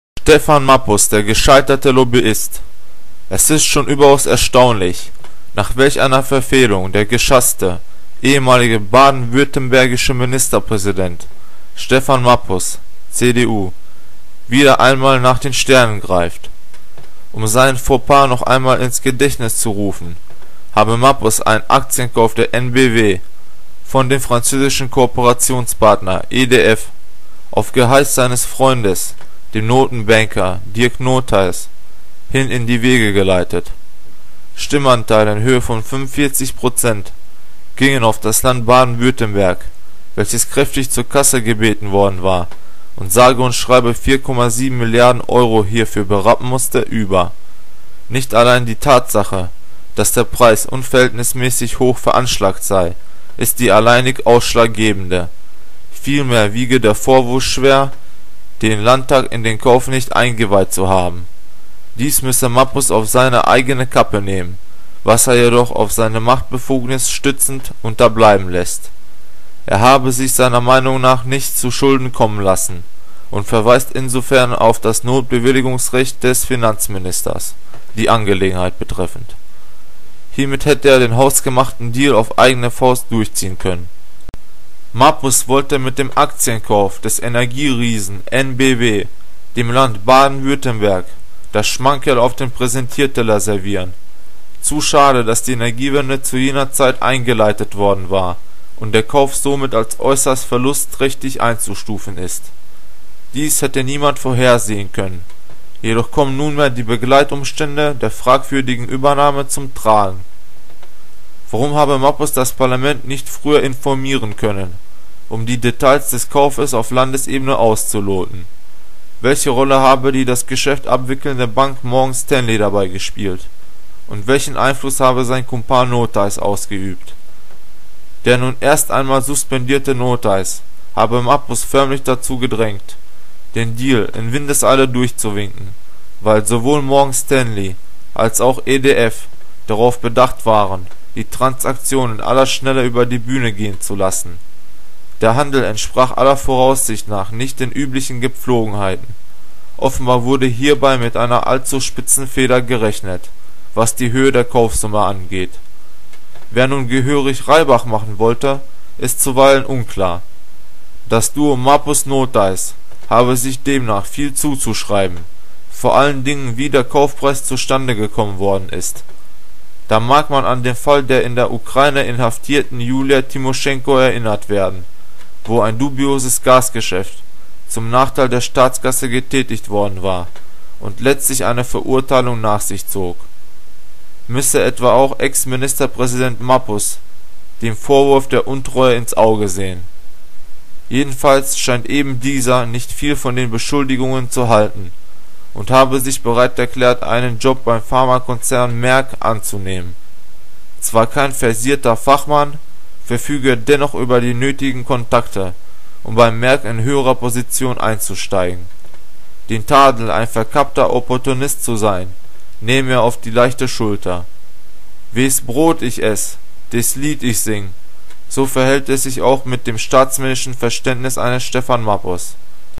Die Causa Mappus
SPRECHROHR- Das ist die Kolumne des Newstwitterdienstes Global News 24.

In der nunmehrigen Auseinandersetzung befassen Wir uns mit dem höchst umstrittenen Deal der Landesregierung Baden-Württembergs, in Person des ehemaligen Ministerpräsidenten Steffan Mappus (CDU) mit dem französischen Konzern EDF um die Kaufanteile eines Aktienpaketes des Stromriesen EnBW. Mappus habe eigenhändig über das Parlament hinweg die Kaufentscheidung getroffen, und muss jetzt Stellung dazu beziehen.